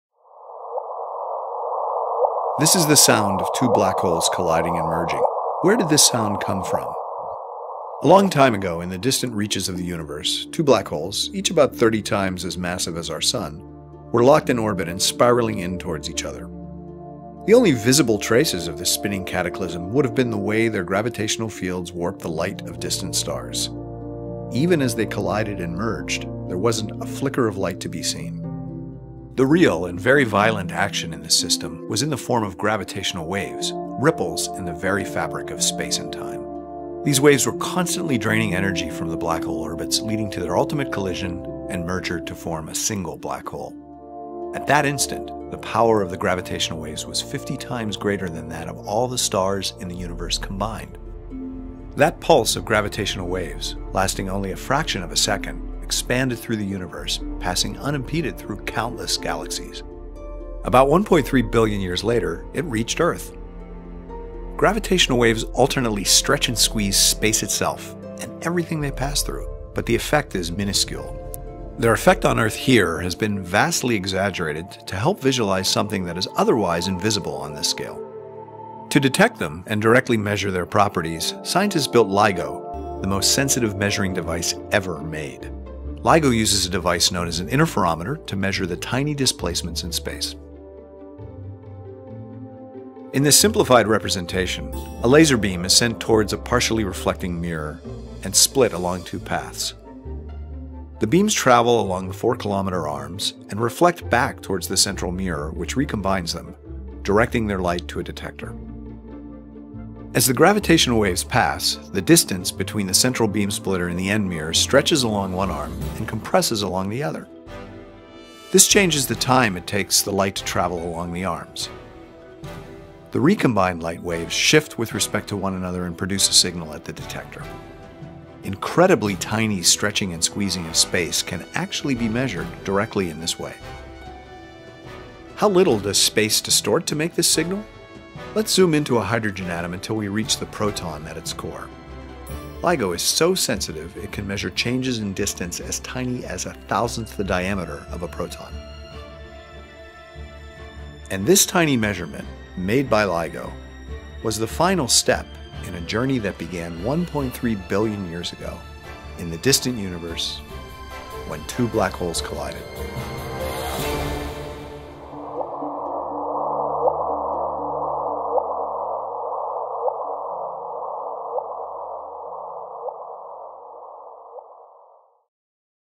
重力波を音に変換して試みた動画 2。２０１６年２月１２日。。
重力波の検出に初成功。